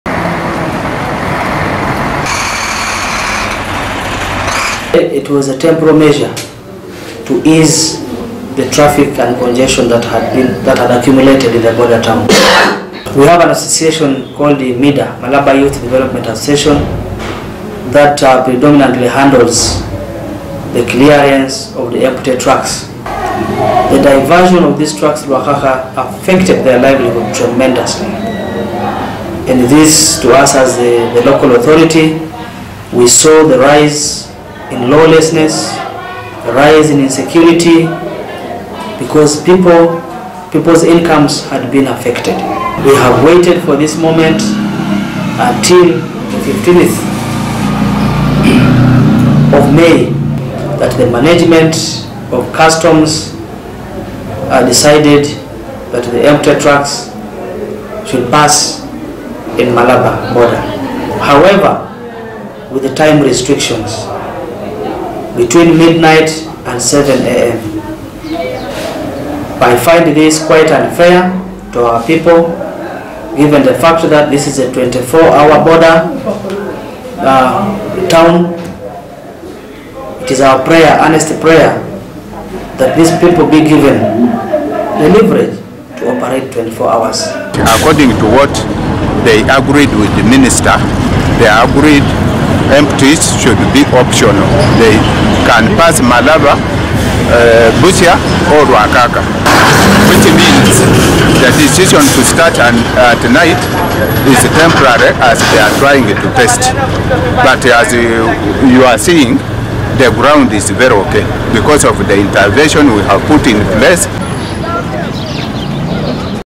Normal cargo flow resumes at Malaba border after security clash
The normal flow of cargo across the Malaba border has resumed following yesterday's clash between Kenyan security and striking clearing agents who had blocked the entry point. By today morning, close to 2000 trucks carrying imports, exports and empty cargo containers had moved across the two borders. Uganda Revenue Authority and Kenya Revenue Authority have indicated that they have lifted the ban on empty trucks, only allowing them to cross the border between 10 pm and 7 am.